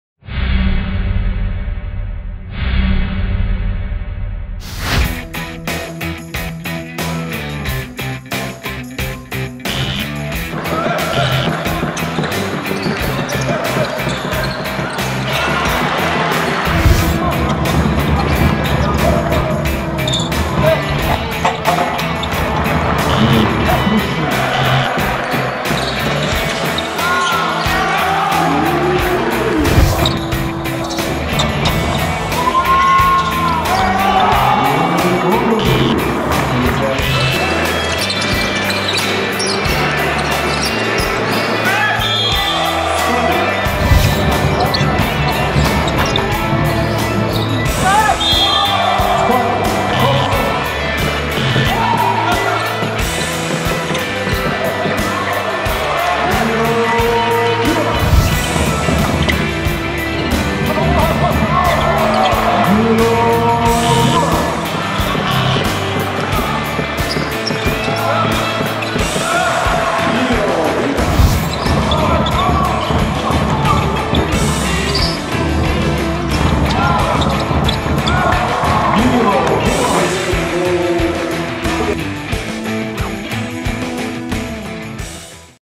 Top 5 I Cedevita - Cibona (9.12.2016.) I KK Cedevita TV
5 najljepših poteza iz utakmice protiv Cibone u 10. kolu A1 lige.